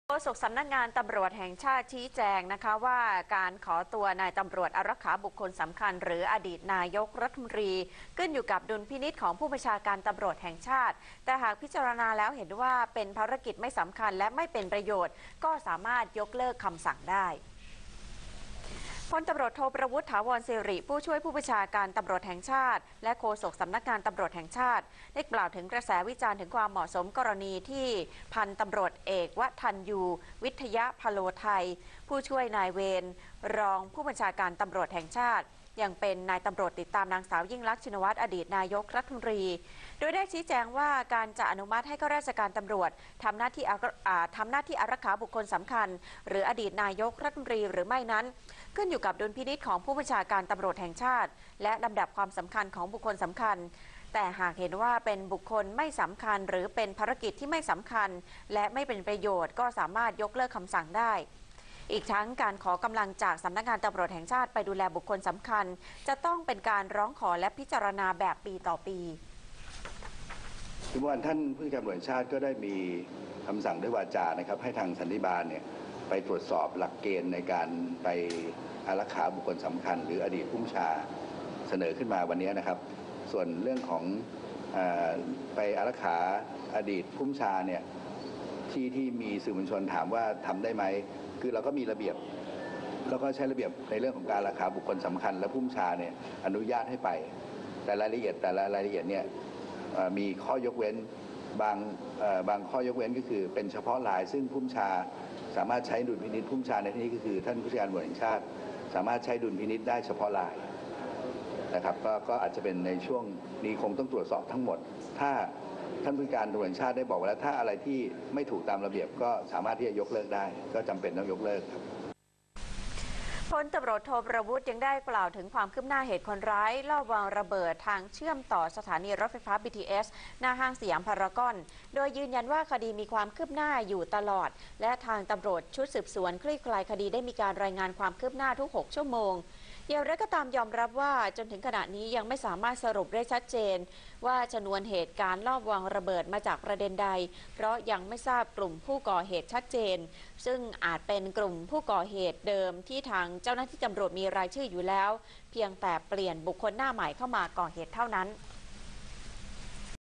โฆษกตร.แจงยกเลิกตำรวจติดตาม "ยิ่งลักษณ์" ได้หากภารกิจไม่จำเป็น
2015/02/18 โฆษกตร.แจงยกเลิกตำรวจติดตาม "ยิ่งลักษณ์" ได้หากภารกิจไม่จำเป็น